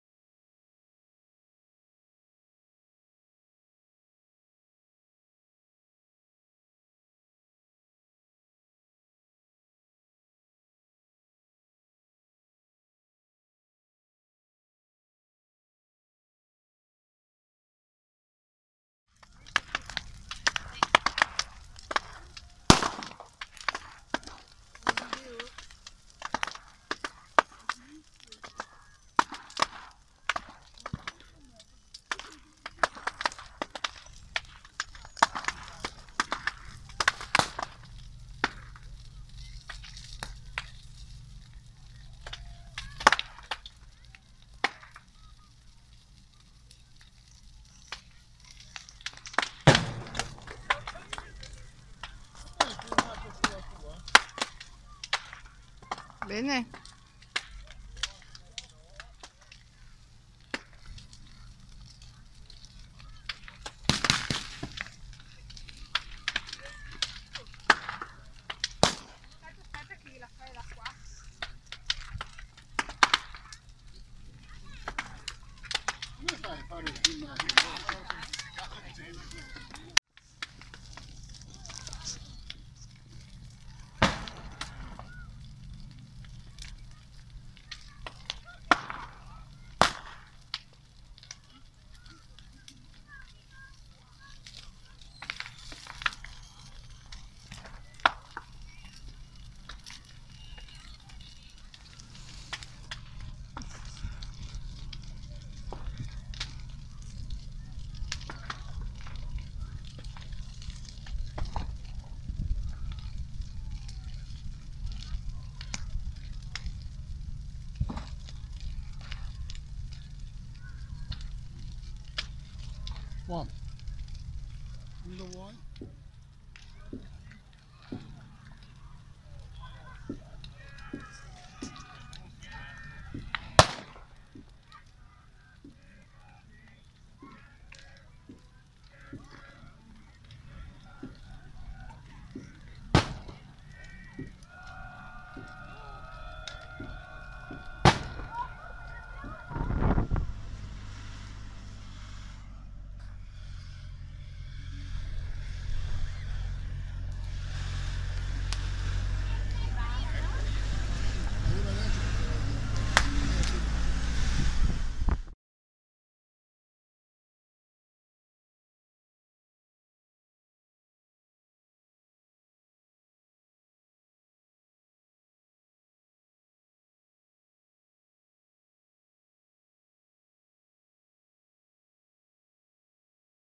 Panevin Grant Castello di Caneva 2017
Due mesi di lavoro per tramandare ai giovani una delle più sentite tradizioni…oltre cento saranno i rimorchi di legno di vite, raccolto in tutto il territorio canevese e accatastato attorno ad un unico palo di sostegno, per creare ancora una volta uno dei più imponenti falò dell’intera Regione. Cerimonia particolare quella del taglio dell’albero, un pioppo di oltre 30 metri, che costituirà lo stollo della grande catasta di legna. Prima dell’accensione, prevista per le ore 20:30, si terrà la cerimonia del Théndro del Thòc de Nadàl, con la dispersione delle ceneri del ceppo natalizio attorno al falò. Ad accensione avvenuta un gruppo di cantori inviterà tutto il pubblico a intonare le antànie ed i motivi più popolari della tradizione locale. Brulé e pintha per tutti! I festeggiamenti avranno luogo in località Spinè.